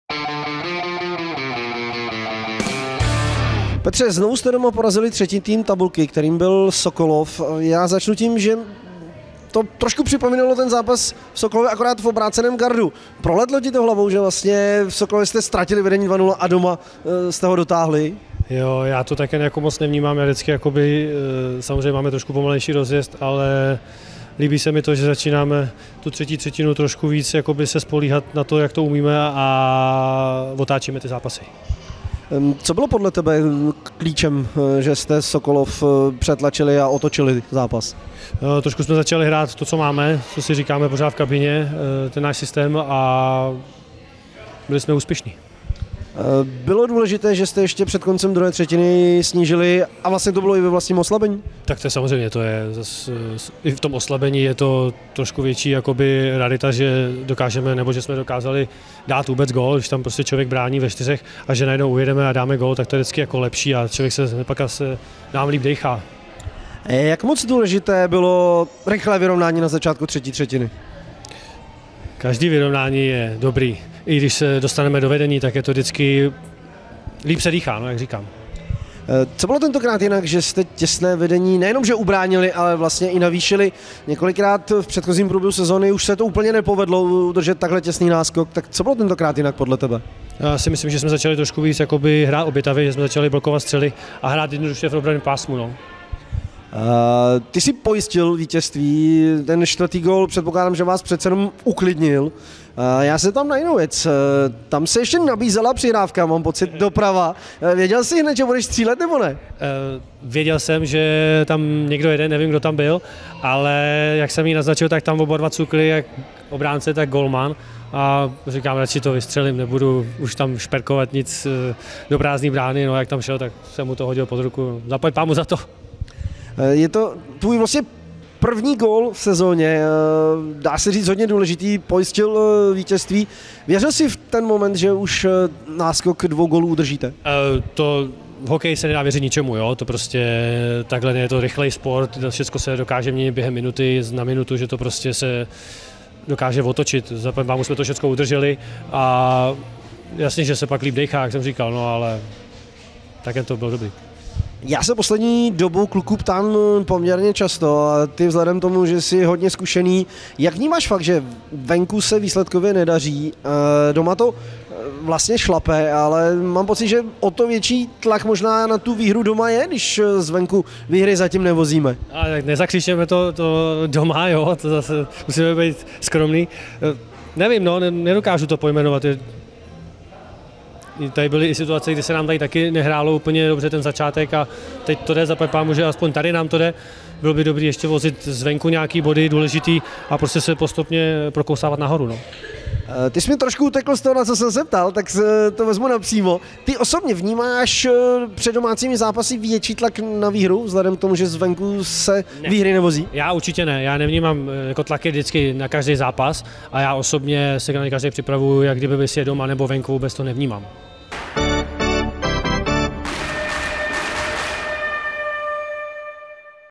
ROZHOVOR 21. kolo Maxa ligy: Petr Ulrych skóroval poprvé v sezóně
Angažmá u Pirátů začal dvěma asistencemi, pak se však soustředil především na defenzivní úkoly. Možná i proto zkušený obránce Petr Ulrych zapsal první bod ročníku 2024/2025 až v šestadvacátém utkání, počítáme-li i přípravu. Jenže ten bod byl hodně důležitý v utkání se třetím Sokolovem, neboť sedmatřicetiletý zadák zvýšil chomutovské vedení na 4:2, což dodalo mužstvu klid po obratu z 0:2. Piráti v závěru náskok ustrážili a získali v ROCKNET ARÉNĚ cenný skalp, po němž Petr Ulrych hovořil o vlastní gólové akci či se zamýšlel nad důvody parádní otočky i zvládnuté koncovky duelu.